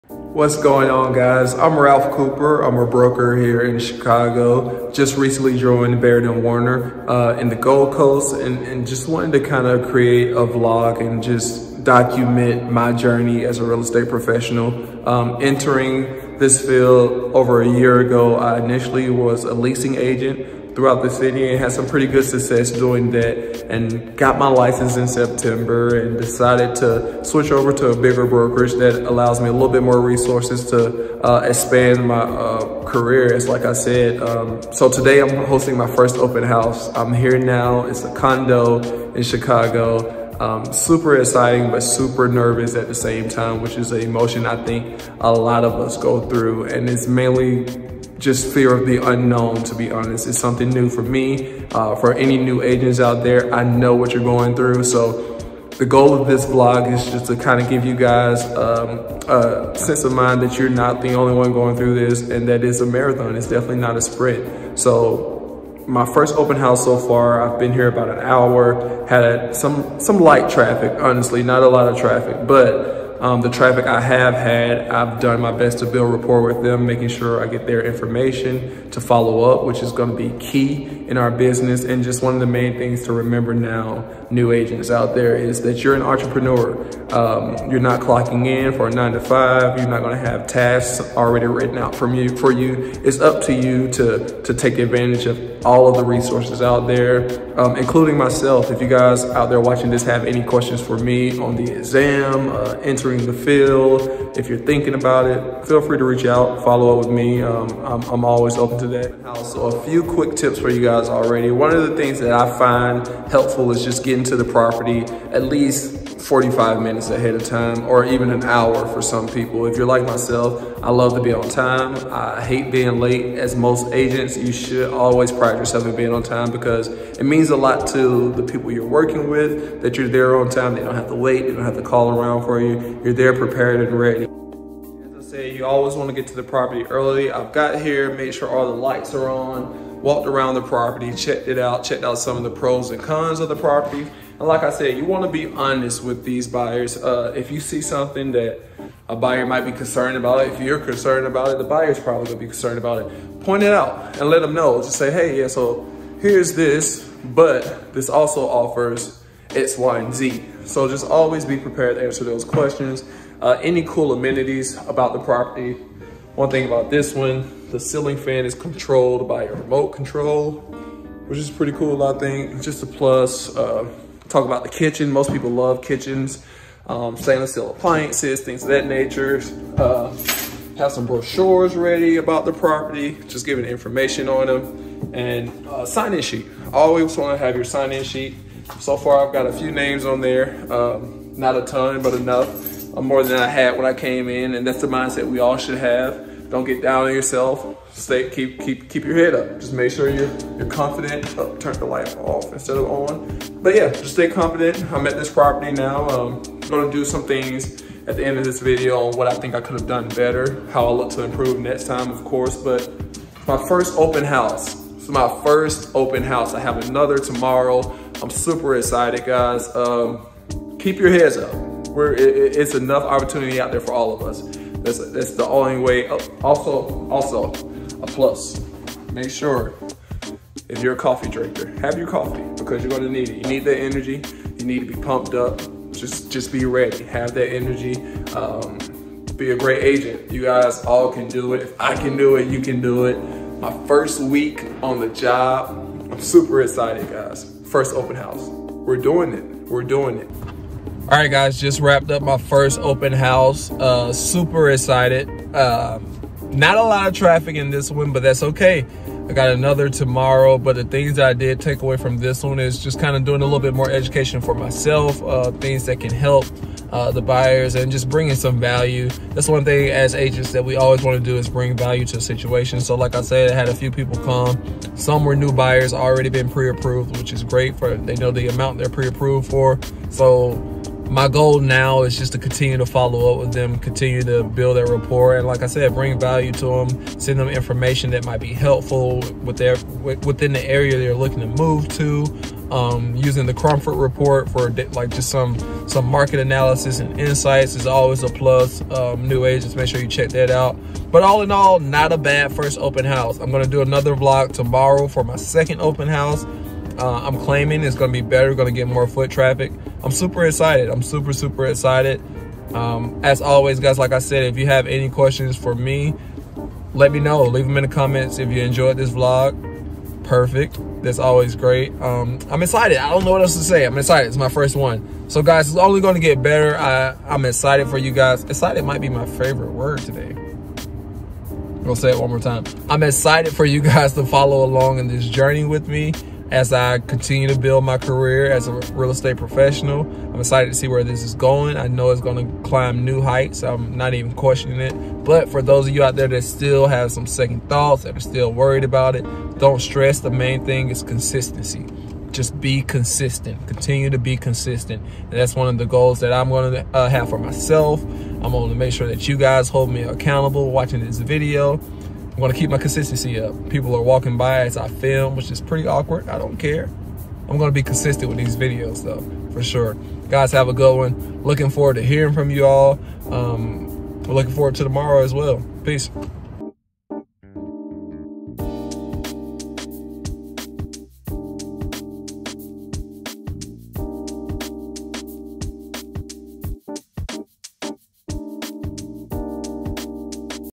My First Open House: Real Estate Journey Vlog #1
As I begin my real estate journey I thought it would be constructive for myself and anyone else out there who's just getting started in real estate or debating whether or not this is the right career for you! I was so nervous but I pushed through and walked away with some great knowledge!

Let me know what you think and if you have any questions for me I'd be happy to help!